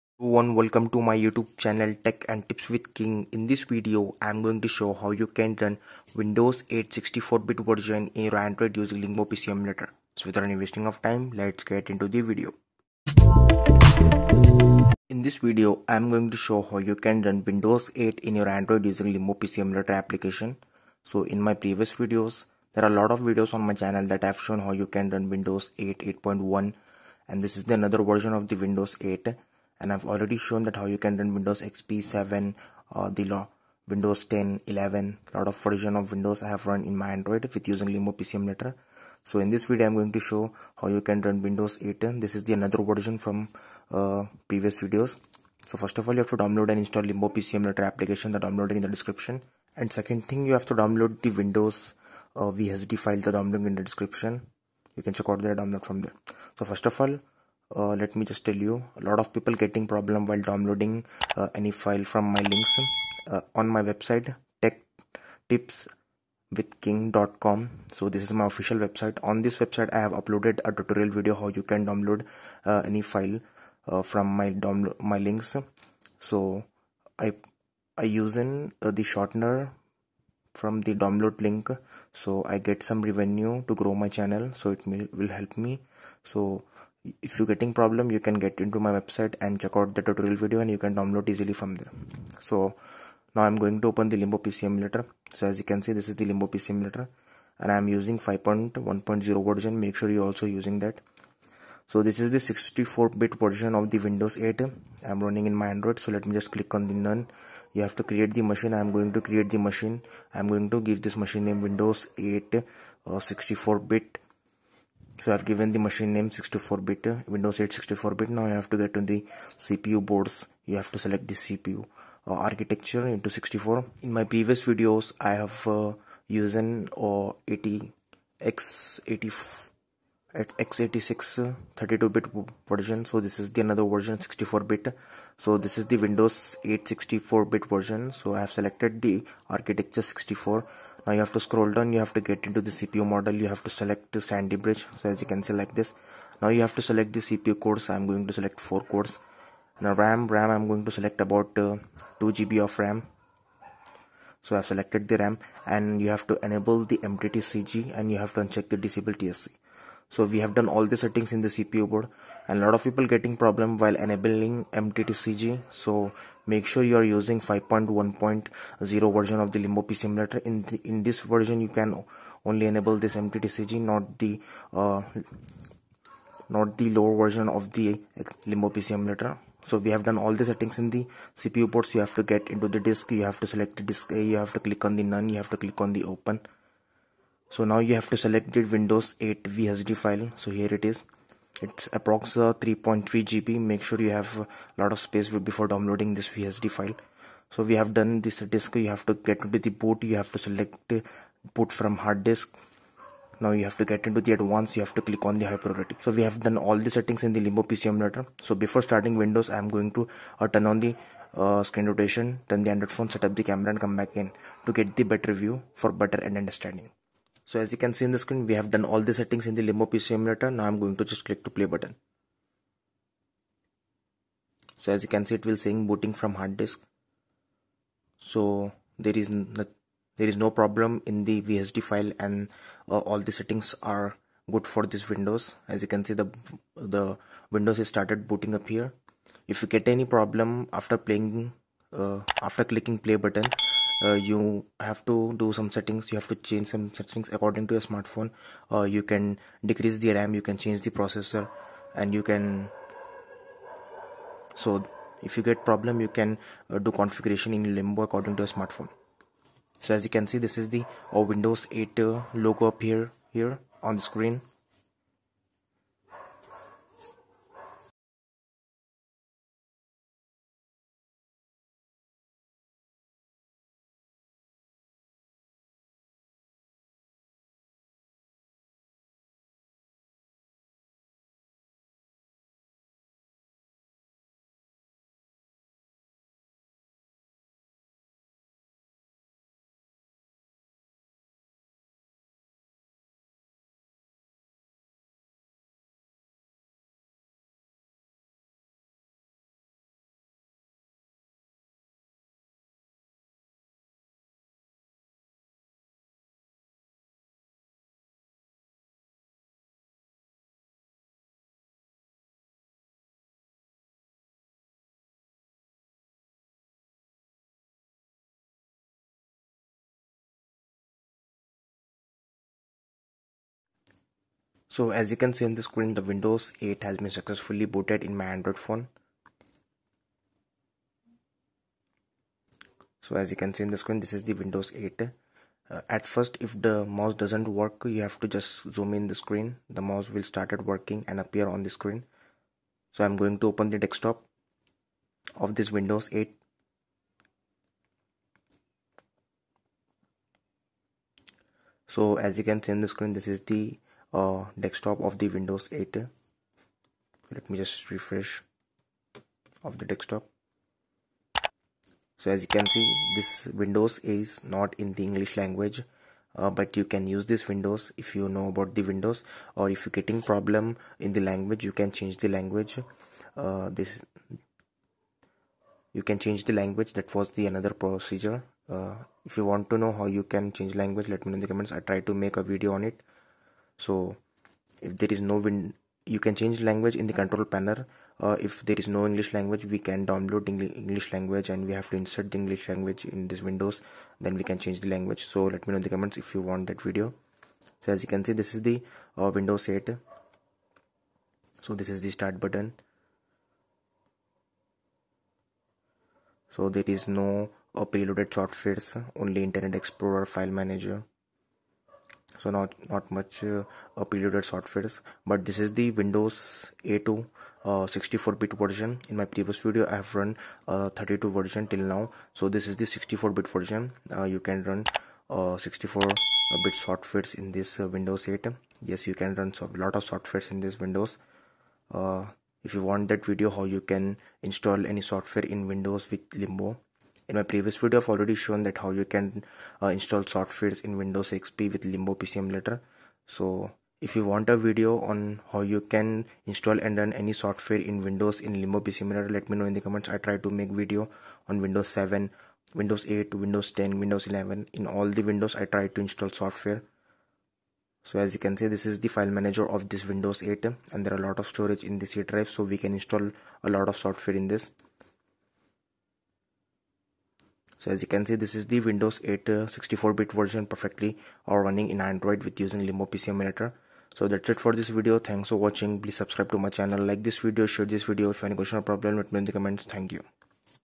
Run Windows 8 64bit in Android Phone Using Limbo PC Emulator
How to Run Windows 8 64bit in Android Smartphone Using Limbo PC Emulator. this is forat time on my YouTube channel that im showing how you can run Windows 8 64bit in Android. a 64 bit windows os in Android. you can install Software's in this Android.